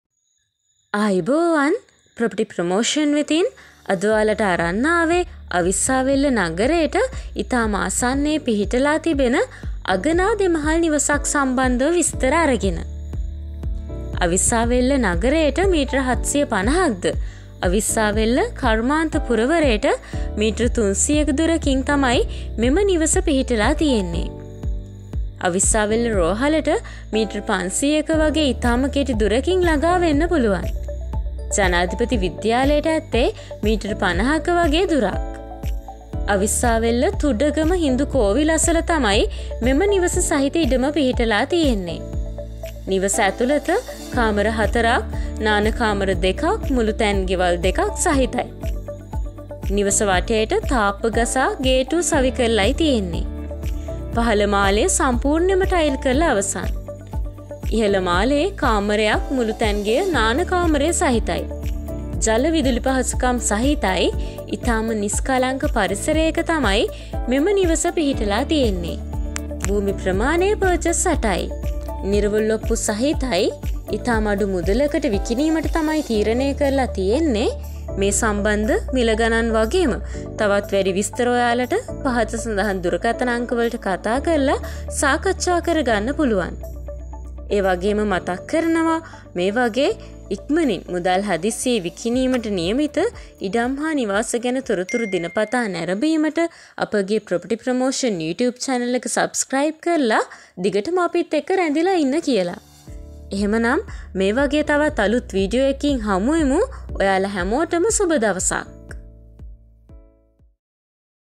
BRN64031: 2 Storey House in Avissawella | 8P | 180Lks | 0716 124 136
අවිස්සාවේල්ල නගරයට ඉතා සමීපව අගනා දෙමහල් නිවසක් ඉතා ඉක්මනින් විකිණීමට

*අවිස්සාවේල්ල නගරයට 750m

*අවිස්සාවේල්ල කර්මාන්ත පුරවරයට 300m

*අවිස්සාවේල්ල රෝහලට 500m

*ජනාධිපති විද්‍යාලයට 50m

*අවිස්සාවේල්ල තුඩගම හින්දු කෝවිල අසල මෙම නිවාස සහිත ඉඩම පිහිටා ඇත,

කාමර 4 ක්

Barth රූම් ,2

මුළුතැන් ගෙවල් 2

*තාප්පය වටේටම ගසා ගේට්ටුවක් සවි කර ඇත.

*පහළ මාලය ටයිල් කර ඇත

*ඉහළ මාලයේ කාමරයක් මුළුතැන්ගෙය,බාත් රූම් ටයිල් කර ඇත

*ජල විදුලි පහසුකම් ඇත

* නිස්කලංක පරිසරය.

* පර්චස් 8 කී

* නිරවුල් ඔප්පු.

*වටිනාකම  ලක්ෂ 180 කී.

*මිළ ගණන් සාකච්ඡා කලා හැක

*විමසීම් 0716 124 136

Property Promotion in Srilanka

◼️ Call for Advertising
~ දැන්වීම් සදහා අමතන්න ~

මුදල් හදිසියේ ඉක්මණින් විකුණන ඉඩම්/නිවාස  පහලින් :
Link එක මත Click කරන්න